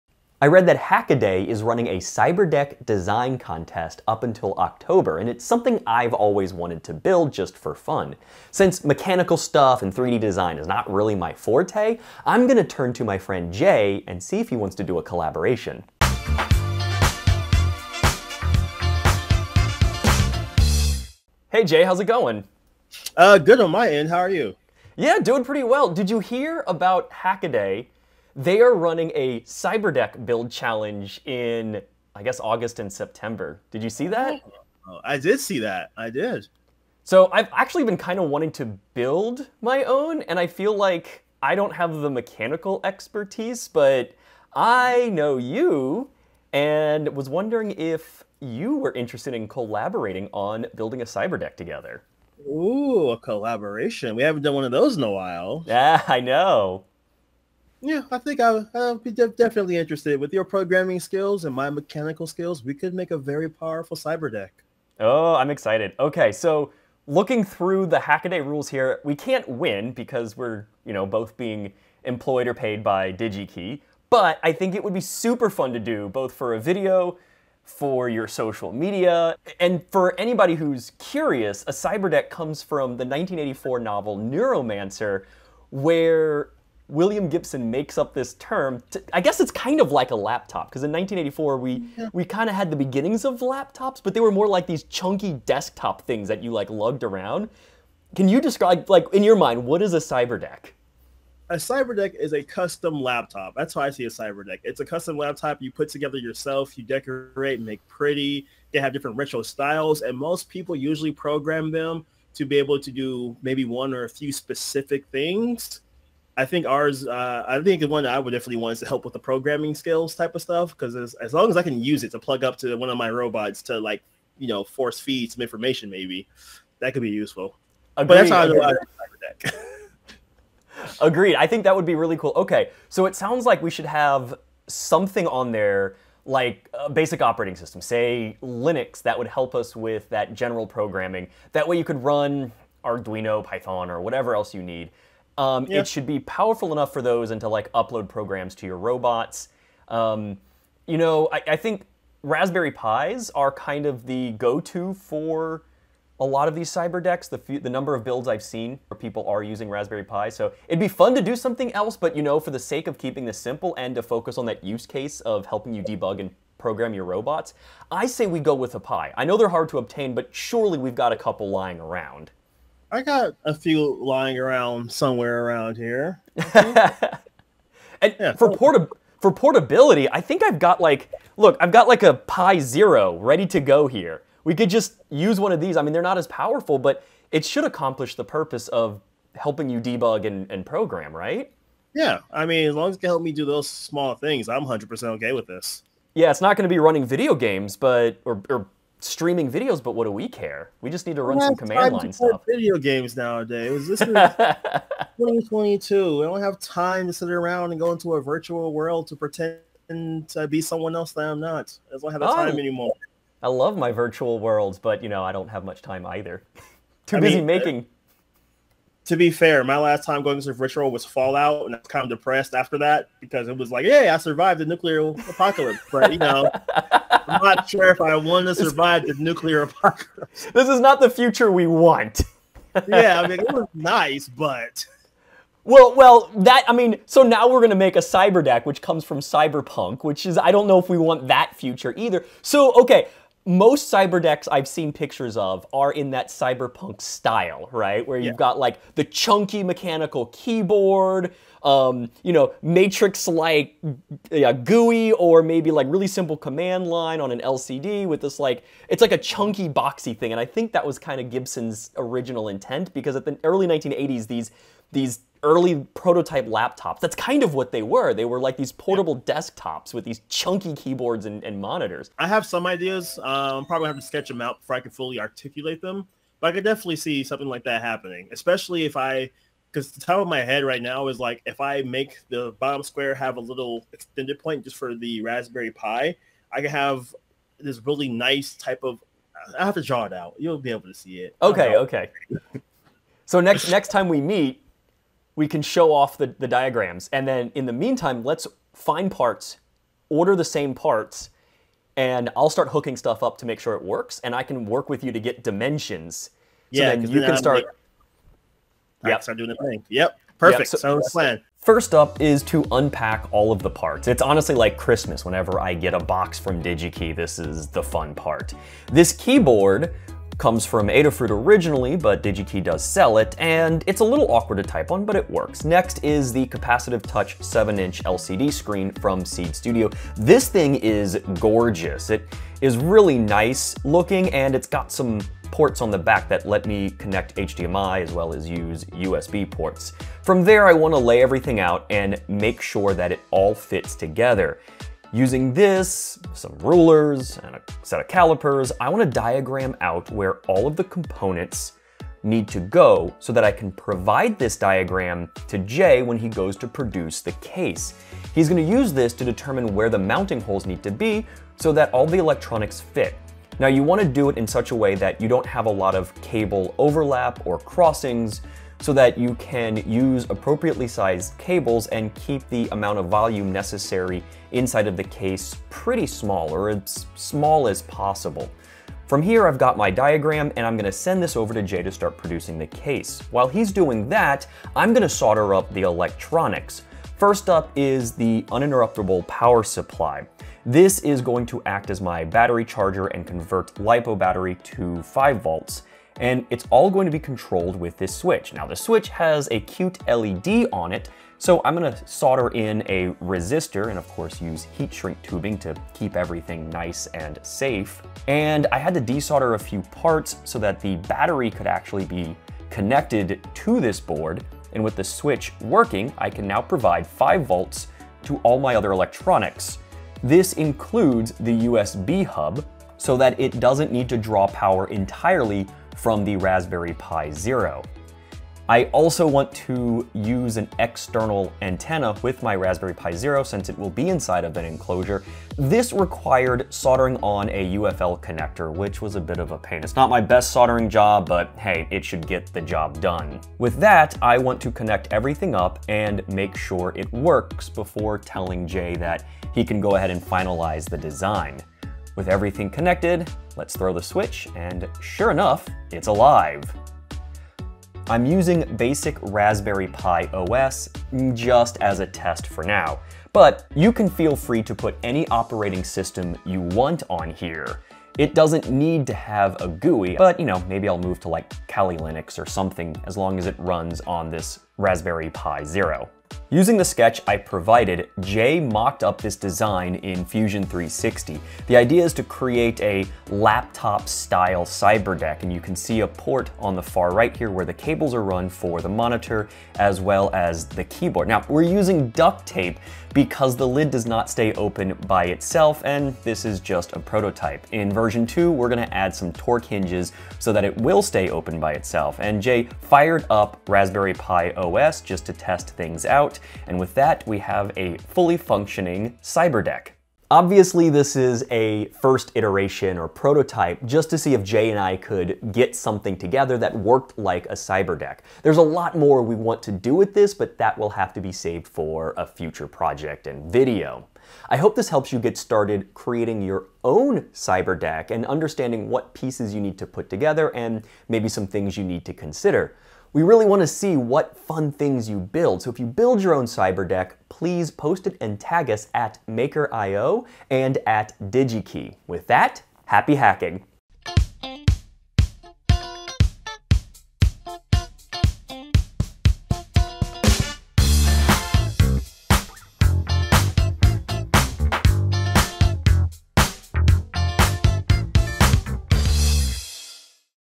Building a Raspberry Pi Cyberdeck | Digi-Key Electronics
William Gibson coined the term “cyberdeck” in his 1984 novel, “Neuromancer.” Just like badgelife, there is a whole group of people designing and building their own cyberdecks for art, fun, and sometimes, useful laptop replacements. In this video, Shawn and Jayy team up to build their own cyberdeck in honor of the 2022 Hackaday CyberDeck Contest

A tutorial with diagrams showing how the electronics were put together can be found

Most custom cyberdeck builds revolve around the Raspberry Pi, although a few folks get creative with other single-board computers and microcontrollers. Jayy and I wanted to keep the build simple so that others could follow along and build upon our design. We used a Raspberry Pi Zero W, as it is small for our mini build and keeps the battery drain low. Additionally, we used a 7-inch touchscreen LCD from Seeed Studio.

By using a Tiny UPS 3.0, we could run the entire build off a single LiPo battery pouch, giving us (theoretically) 2-3 hours of runtime. However, I noticed the Tiny UPS would reset the Pi whenever the USB power cable was connected or disconnected. The workaround was to make sure to unplug the AC adapter first before unplugging the USB cable or to plug in the USB cable first before plugging in the AC adapter.

While the tiny keyboard was not the most pleasant to type on, it kept the build very small, creating something equivalent to an 8 or 9-inch laptop. This was perfect for such a cyberdeck that could be carried around in a smaller pouch. Jayy wanted to have a small computer capable of making field repairs on his robots, and this cyberdeck fit the bill perfectly.

While the onboard 2.4 GHz antenna would work on the Pi Zero, it would likely be quite limited, as the entire board was enclosed (albeit plastic, but it’s still not great). To help remedy this, we had to add an external antenna to the Pi Zero by attaching a u.FL connector. This guide offers a great walkthrough on how to perform that hack.

This is the first iteration of the design, and we were quite happy with how it came out. We hope that it inspires you to build your own cyberdeck and maybe enter a future contest!